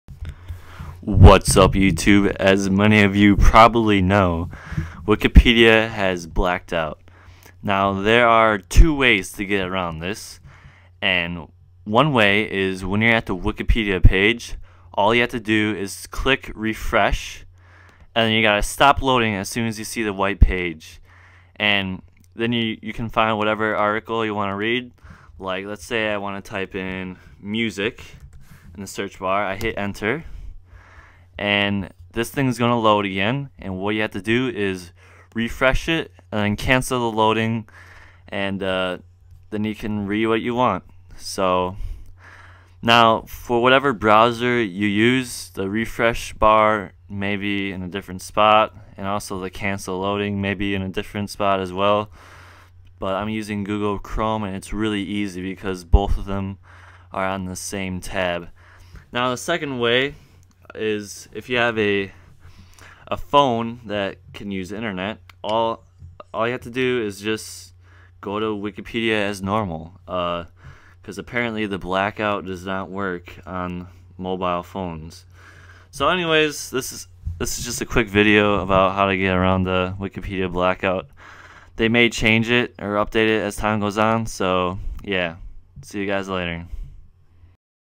How to Get Past Wikipedia Blackout
3 Ways

- Cancel loading the page as soon as you see the correct document

- Use a mobile device

- Disable Javascript